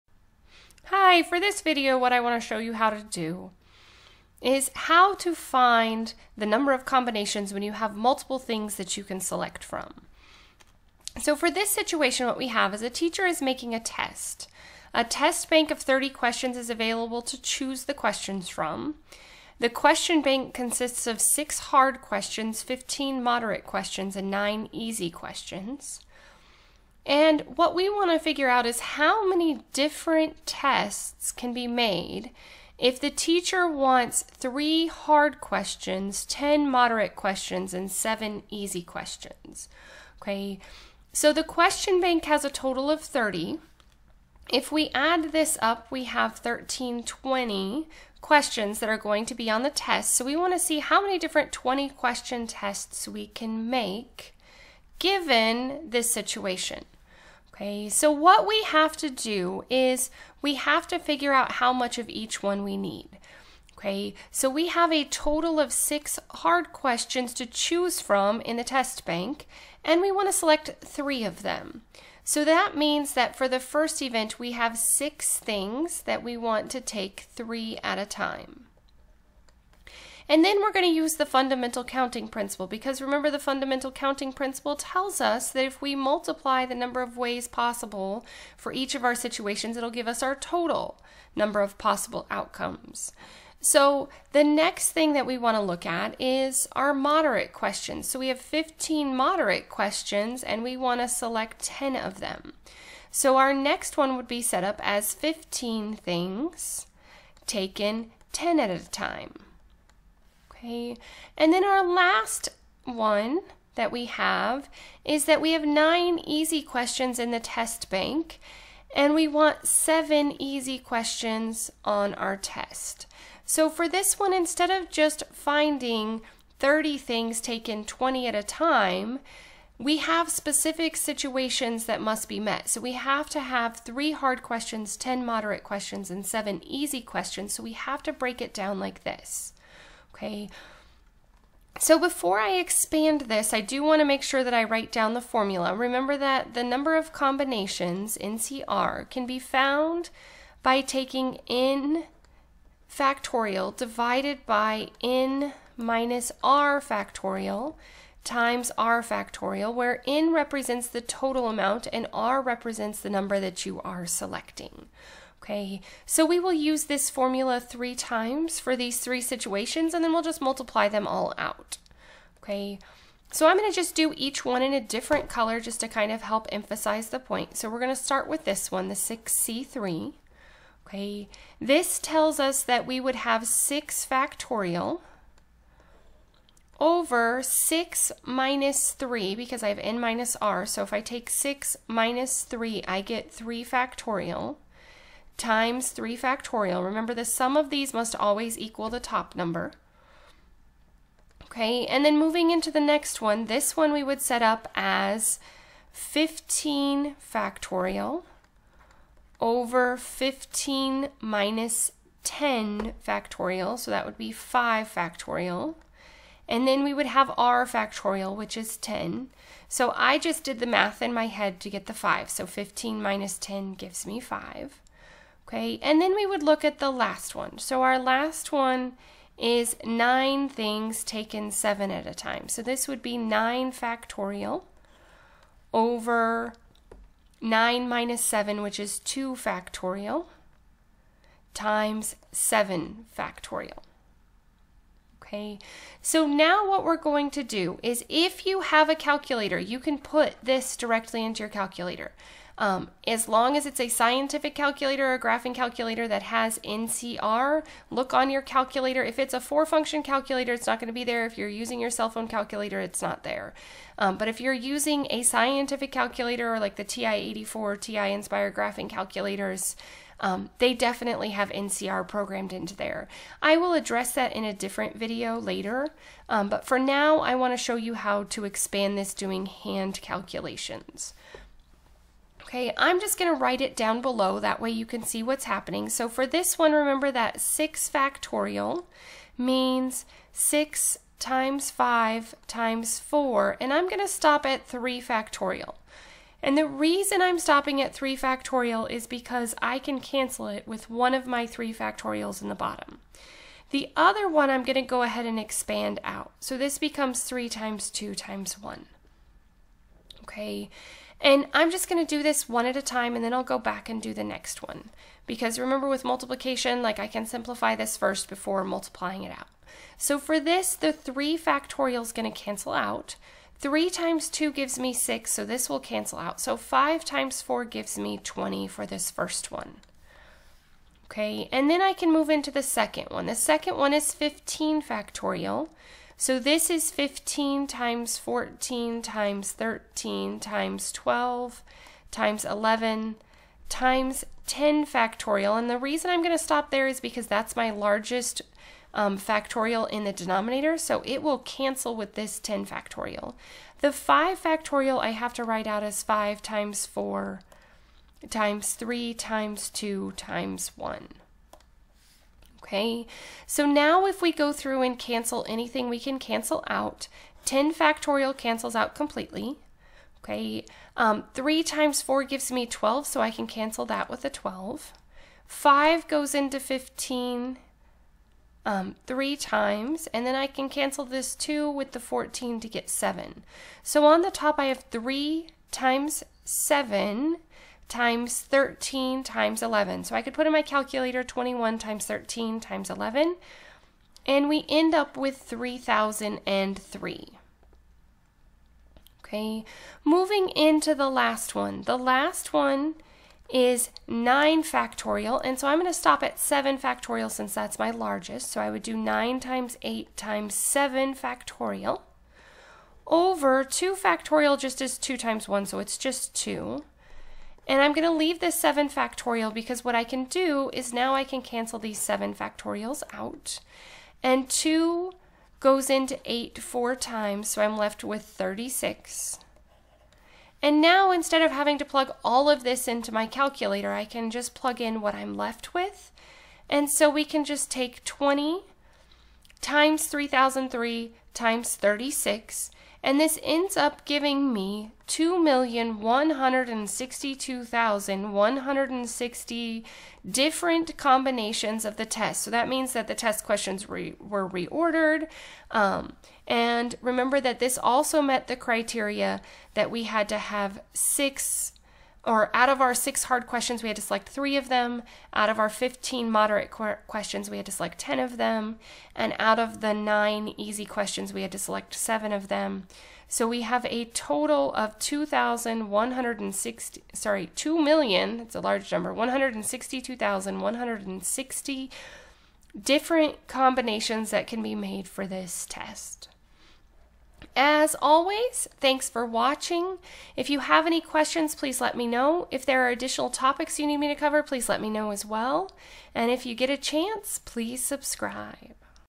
Multiple Combinations
This video addresses what to do if you have multiple items that you are taking combinations of. It basically blends the fundamental counting principal with the combination formula.